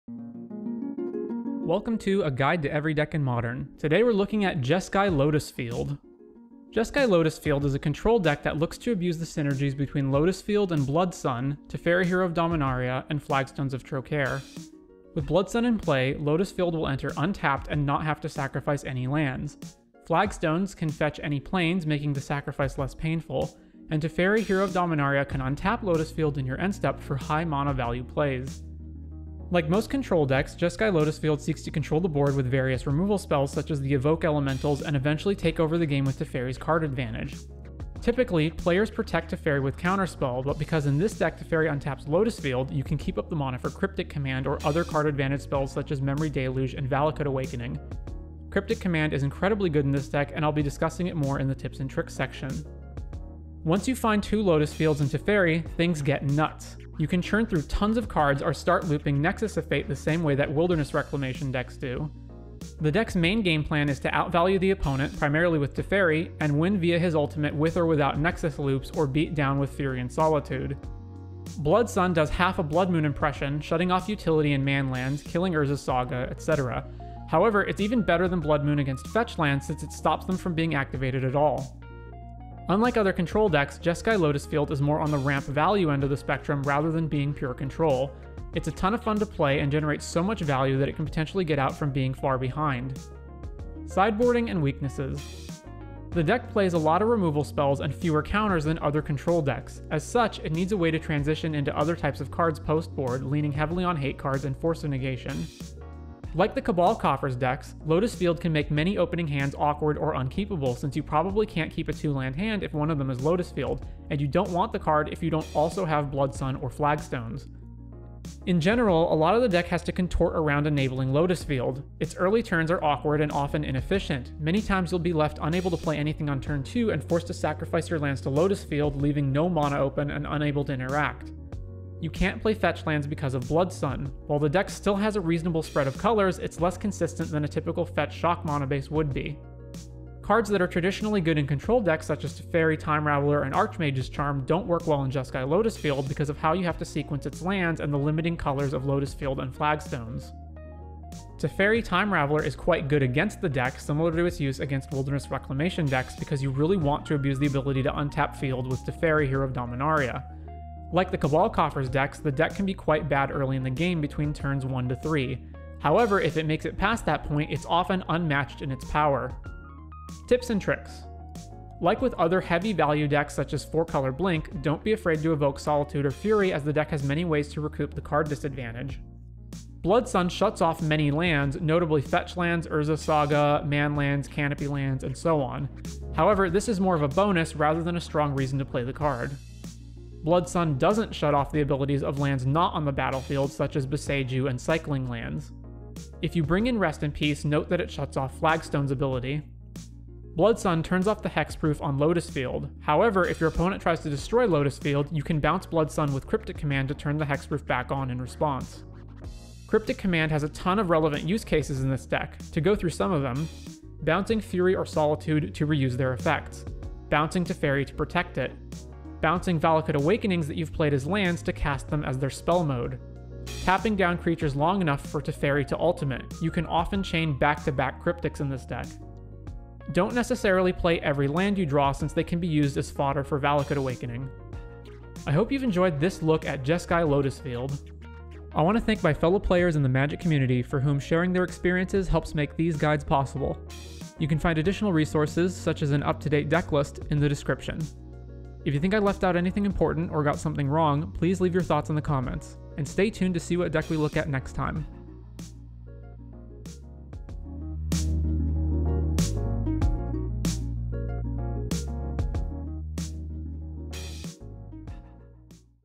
Jeskai Lotus Field | A Guide To Every Deck In Modern
Welcome to A Guide To Every Deck In Modern. Today we're taking a look at Jeskai Lotus Field.

Animated Background - Vimeo Free Videos

0:00 Overview
1:33 Sideboarding & Weaknesses
2:49 Tips & Tricks
3:54 Outro